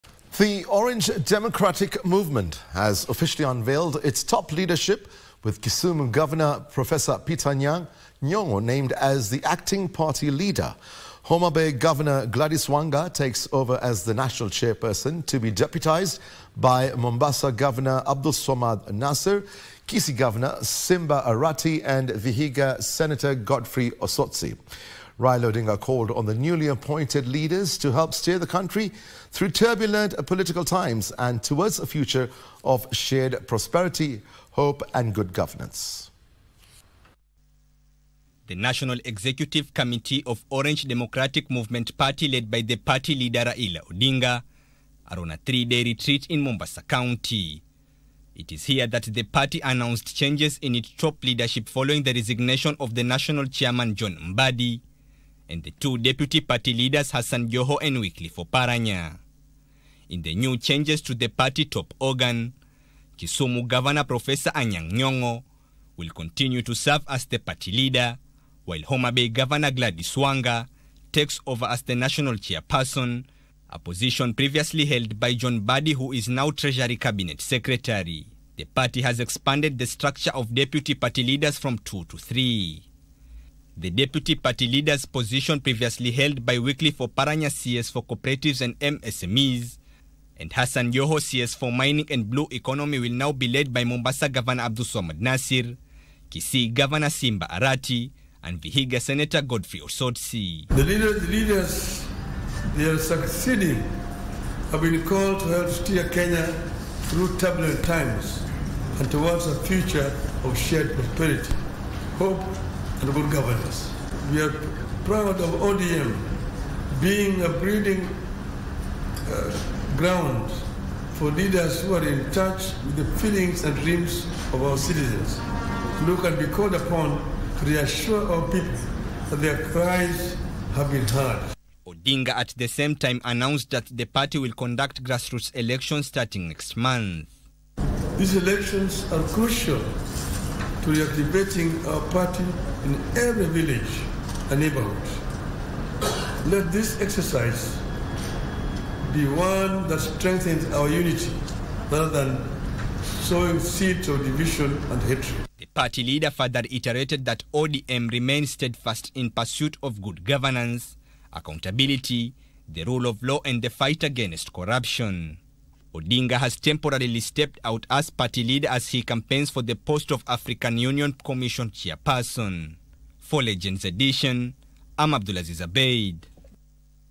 ODM unveils its top leadership
The Orange Democratic Movement has officially unveiled its top leadership with Kisumu Governor, Prof. Peter Anyang’ named as the Acting party leader. Homabay Governor Gladys Wanga takes over as the National chairperson to be deputized by Mombasa governor Abdulswamad Nassir, Kisii governor Simba Arati and Vihiga senator Godfrey Osotsi. Raila odinga called on the newly appointed leaders to help steer the country through turbulent political times and towards a future of shared prosperity, hope, and good governance.